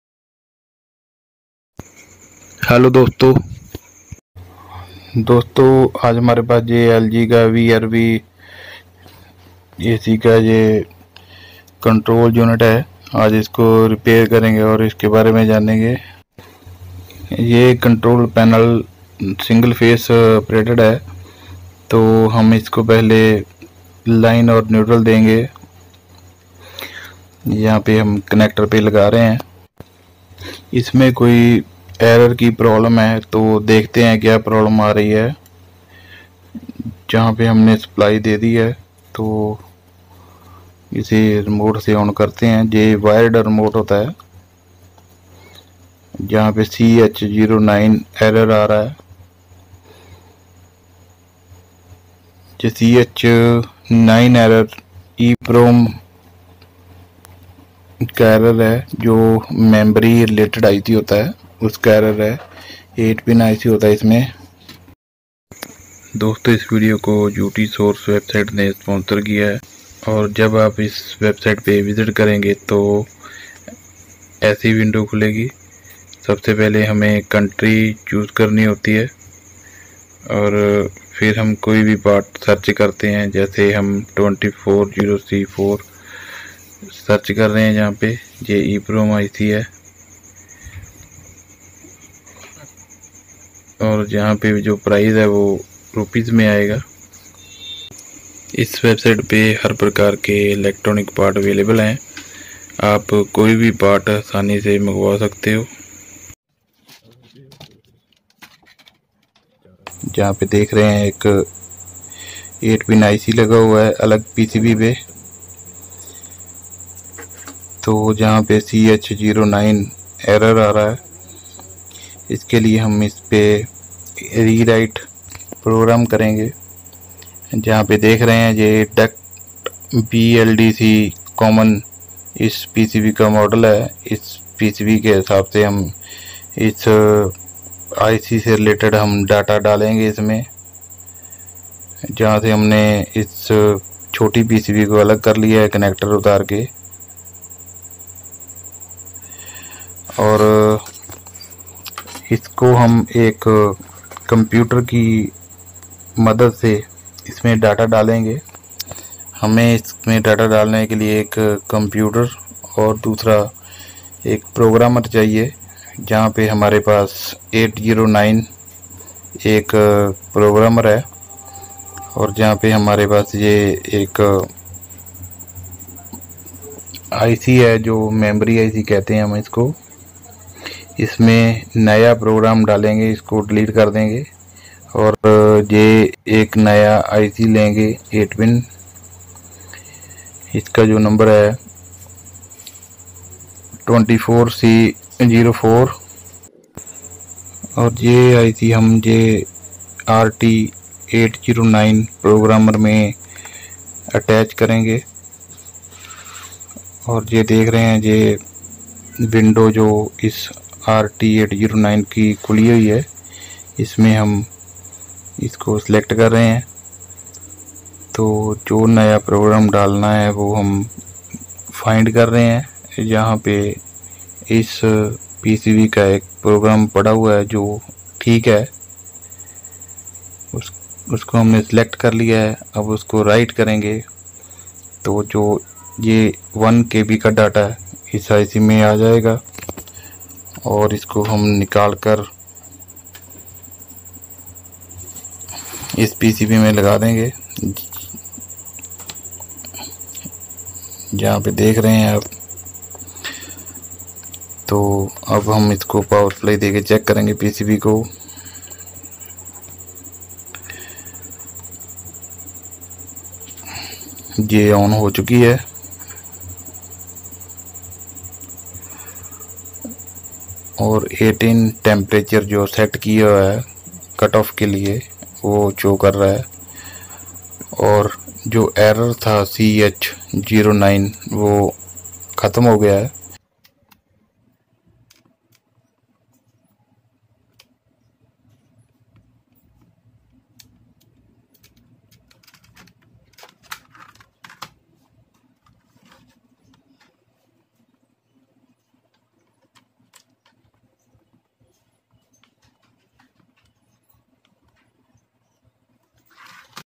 LG VRV cooling Unit's control unit repair & tips(CH 9)
In this video, we have explained about  LG vrv control unit's CH 9 error, how to solve it's